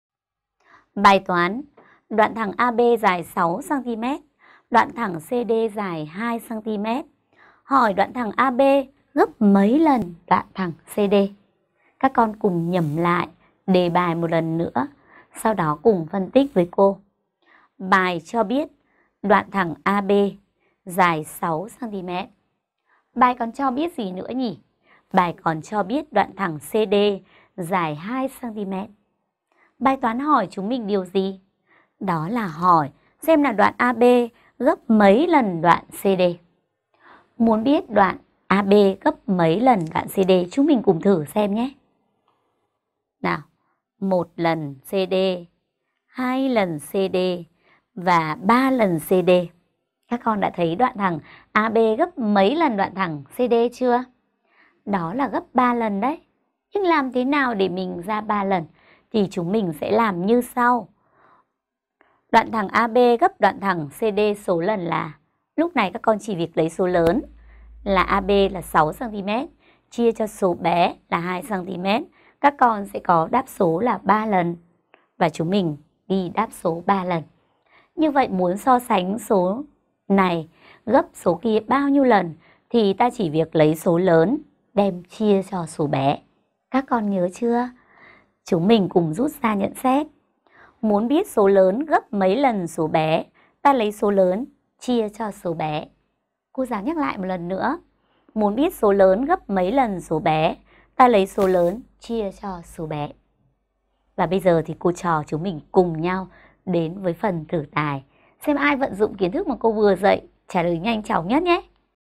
So sánh hai số gấp nhau số lần - Toán 3
Bài toán: Đoạn thẳng AB dài 6cm, đoạn thẳng CD dài 2cm. Hỏi đoạn thẳng AB gấp mấy lần đoạn thẳng CD?
Bài giải
Đoạn thẳng AB gấp đoạn thẳng CD số lần là:
6 : 2 = 3 (lần)
Đáp số: 3 lần
NHẬN XÉT:
Muốn biết số lớn gấp mấy lần số bé ta lấy số lớn chia cho số bé